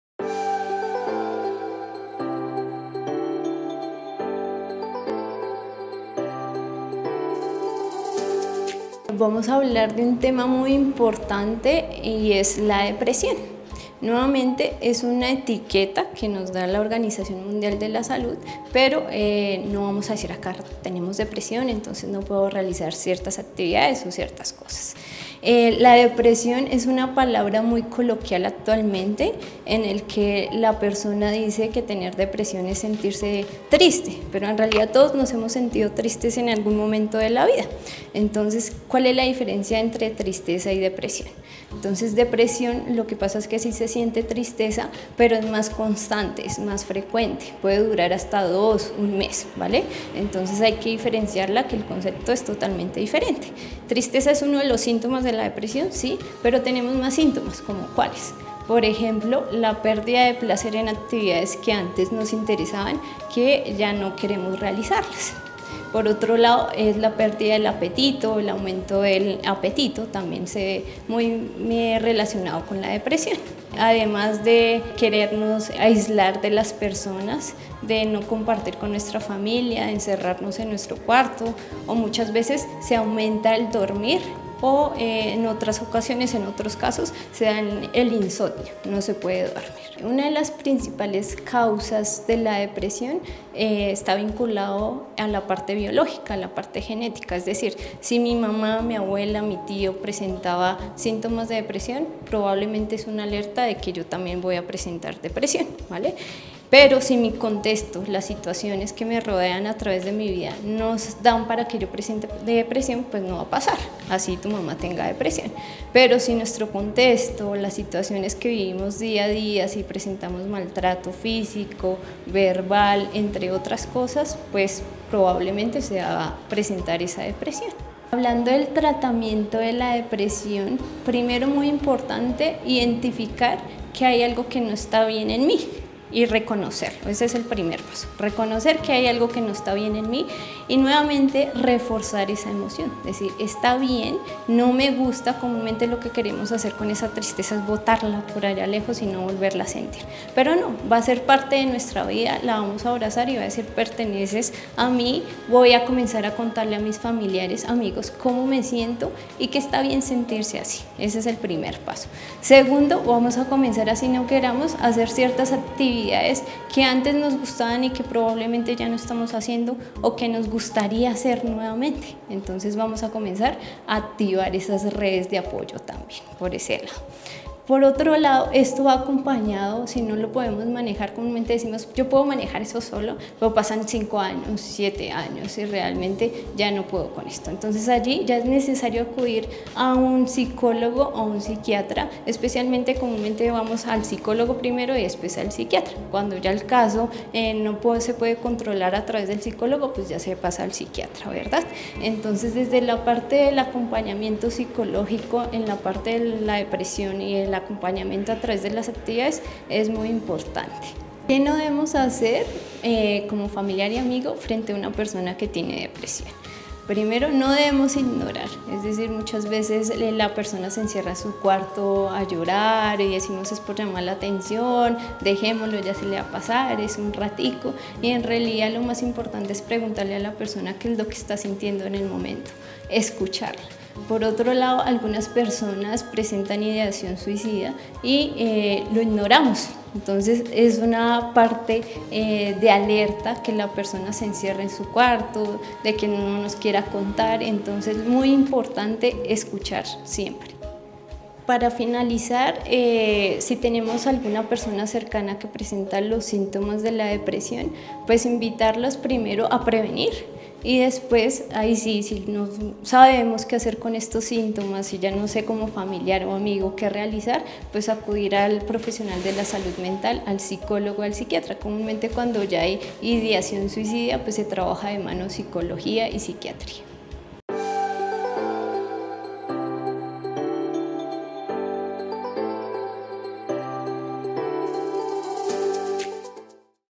Síntomas y Tratamiento de la Depresión
SanaMente en Extrategia Medios
La doctora Deisy Ocampo del Centro de Psicología Deisy Ocampo nos habla acerca de la depresión, cuáles son sus síntomas y tratamientos.

🕑 Entérate:
00:00 – 00:53 Definición y diferencia de la tristeza
00:54 – 01:30 Síntomas de la depresión
01:31 – 02:07 Causas de la depresión
02:08 – 02:58 Tratamiento de la depresión

✅ Agradecimientos:
Centro de Psicología Deisy Ocampo.